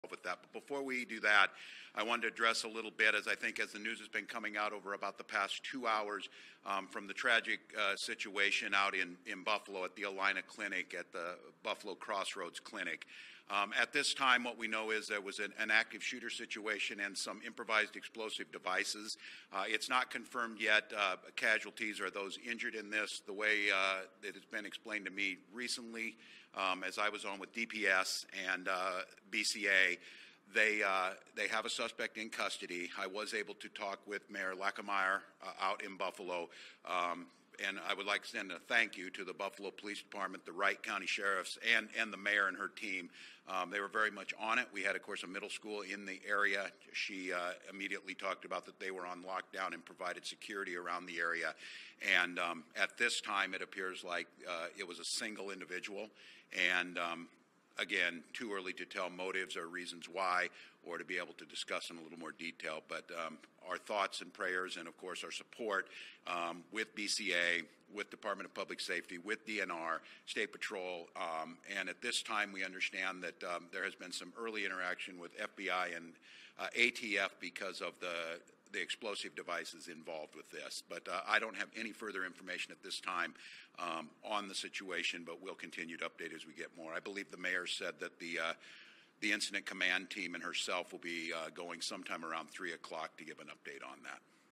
Gov. Tim Walz comments on Buffalo, MN clinic shooting
Governor Tim Walz addresses a shooting at a clinic in Buffalo, MN, ahead of a scheduled vaccine event on Tuesday, Feb. 9.